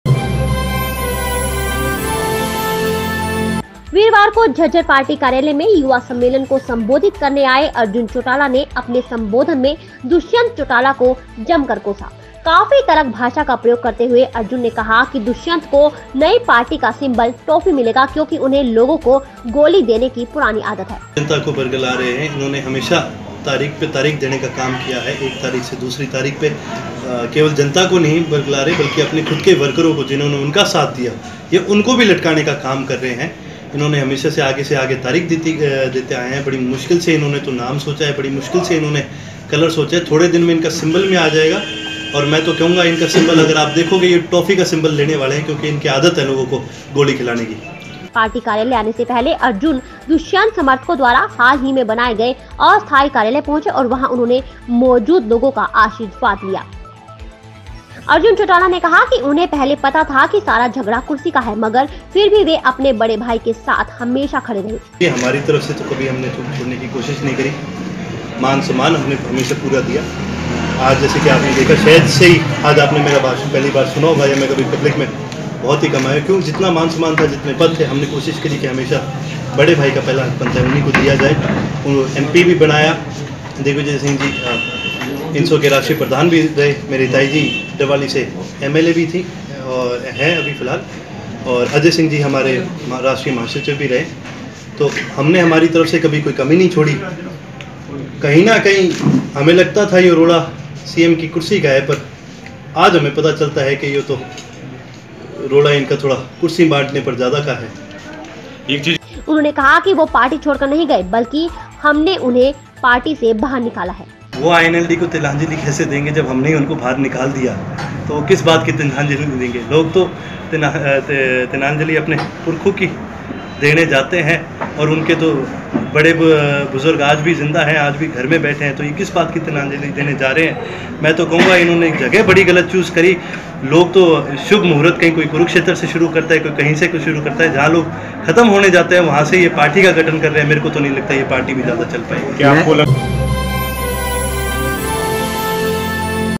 अर्जुन के गांडीव से दुष्यंत के लिए निकले तीखे तीर बोले झगड़ा सीएम कुर्सी का
वीरवार को झज्जर पार्टी कार्यालय में युवा सम्मेलन को संबोधित करने आये अर्जुन चौटाला  ने अपने संबोधन में दुष्यन्त चौटाला को जमकर कोसा। काफी तल्ख भाषा का प्रयोग करते हुए अर्जुन ने कहा कि दुष्यन्त को नई पार्टी का सिम्बल टॉफ़ी मिलेगा क्योंकि उन्हें लोगों को गोली देने की पुरानी आदत है। पार्टी कार्यालय आने से पहले अर्जुन दुष्यन्त समर्थकों द्वारा हाल ही बनाये गए अस्थाई कार्यालय पहुंचे और वहां उन्होंने मौजूद लोगों का आर्शीवाद लिया।

अर्जुन चौटाला ने कहा कि उन्हें पहले पता था कि सारा झगड़ा कुर्सी का है मगर फिर भी वे अपने बड़े भाई के साथ हमेशा खड़े रहे। उन्होंने कहा कि वो पार्टी छोड़कर नही गए बल्कि हमने उन्हें बाहर निकाला। वे कहते हैं कि हमने पार्टी को तिलांजलि दे दी, कोई उन्हें समझाए की तिलांजलि पुरखों को दी जाती है और उनके पुरखे अभी घर बैठे हैं।